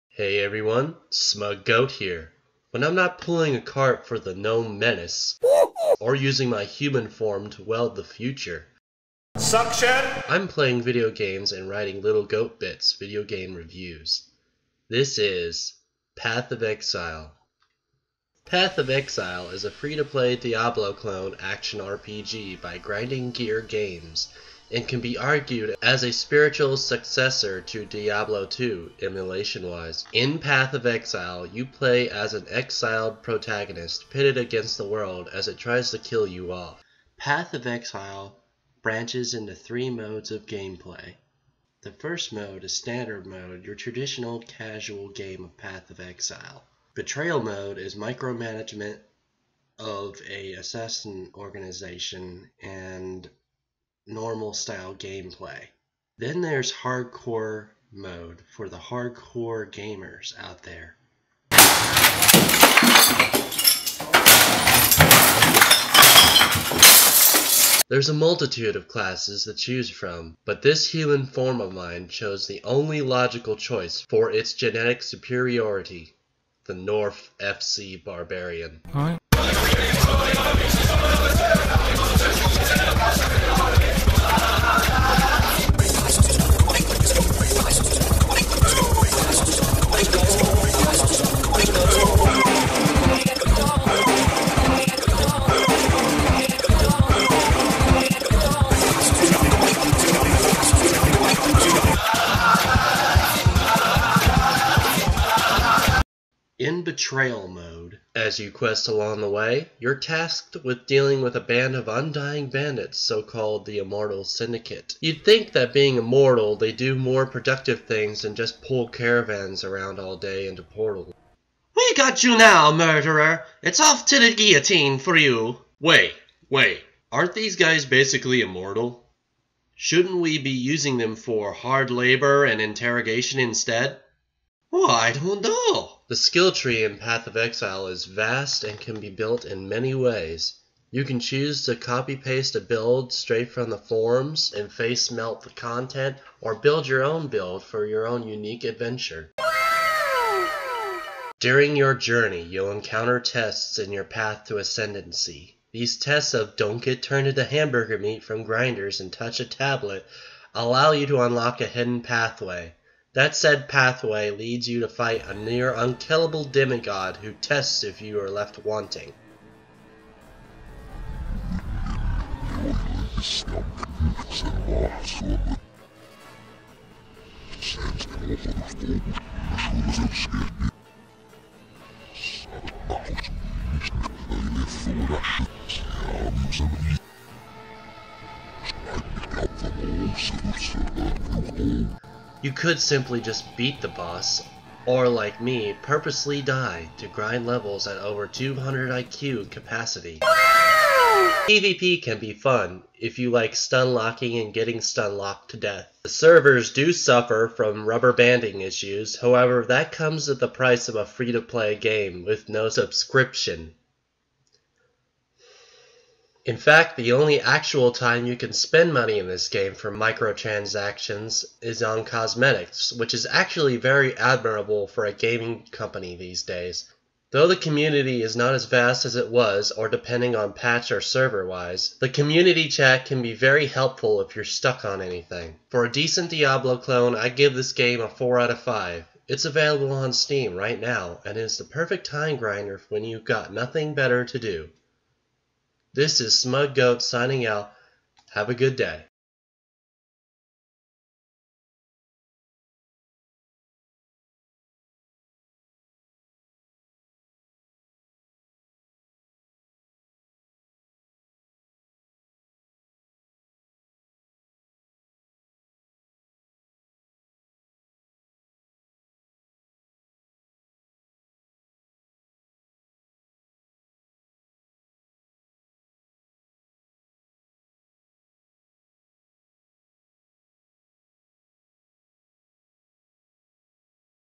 Lil' Goat Bits Youtube Episode 4
In this anticipated episode, I review the free-to-play game Path of Exile.

I felt it's a little rushed, but it's a good video for me to get back into the upload game. ( Will be improving every video upload)